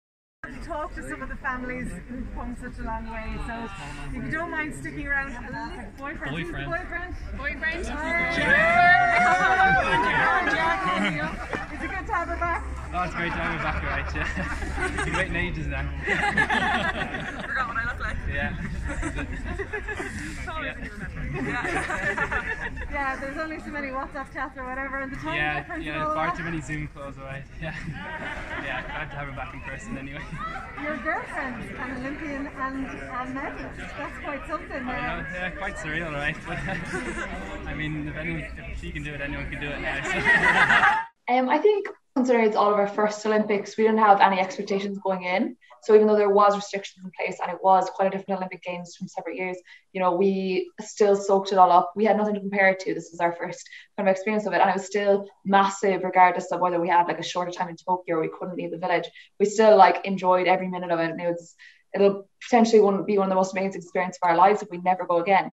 Ireland's bronze-winning rowers welcomed home by families
Ireland’s Olympic rowing medalists have returned home from Tokyo, with women's four bronze medal-winner Emily Hegarty telling a press conference that they "enjoyed every minute" of their sporting odyssey.